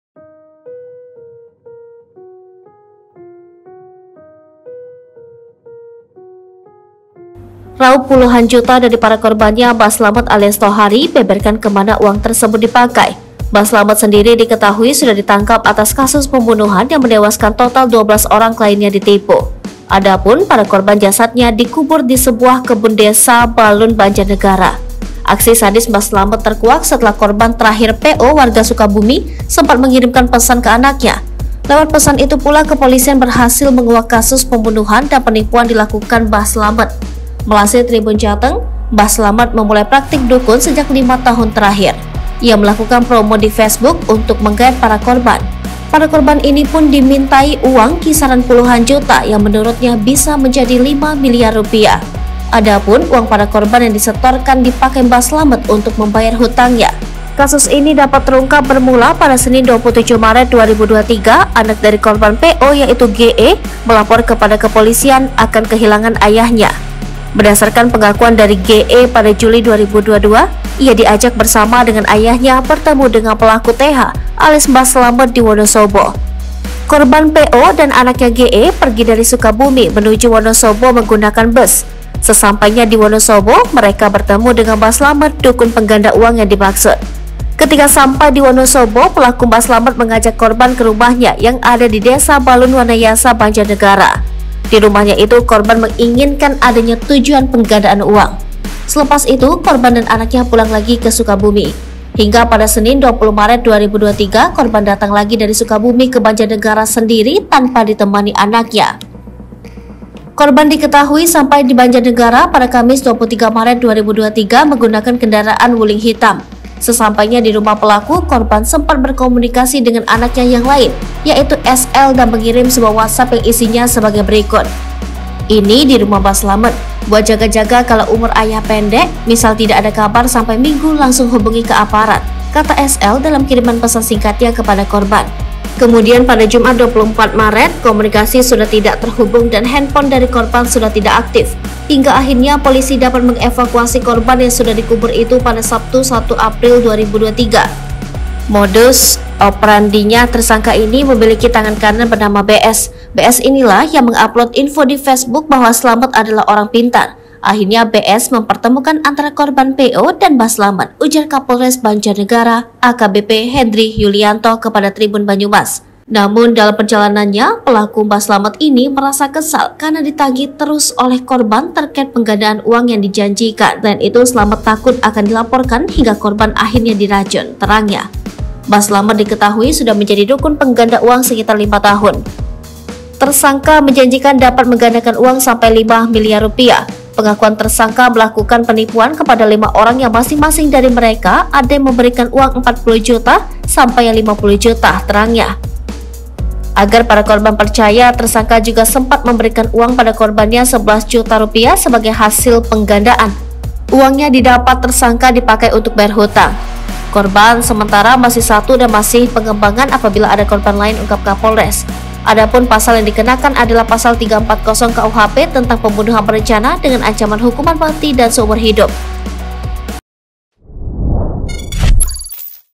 Tega Tipu dan Tewaskan 12 Orang, Mbah Slamet Janjikan Gandakan Uang dari Puluhan Juta Jadi Milliaran
Raup puluhan juta dari para korbannya, Mbah Slamet alias Tohari (45) beberkan kemana uang tersebut dipakai.

Mbah Slamet sendiri diketahui sudah ditangkap atas kasus pembunuhan yang menewaskan total 12 orang kliennya ditipu.

Adapun para korban jasadnya di kubut di sebuah kebun desa balun Banjarnegara.

Aksi sadis Mbah Slamet terkuak setelah korban terkahir PO warga Sukabumi sempat mengirimkan pesan ke anaknya.

Lihat informasi terkini bersama kami, di:

Palembang.tribunnews.com